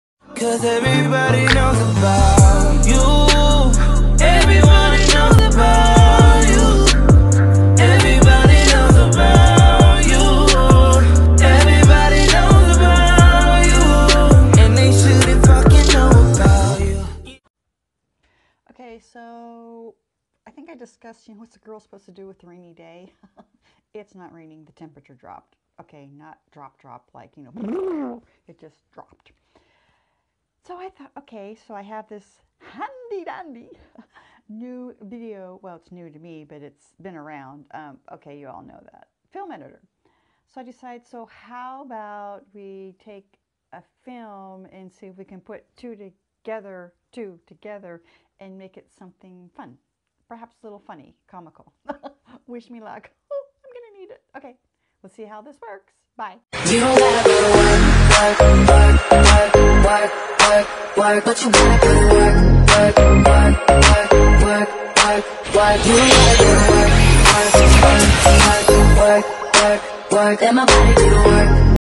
Let's use the editing feature.
Sarah chose to broaden her video skills. She placed a couple of other videos into this one. A work in progress. 😁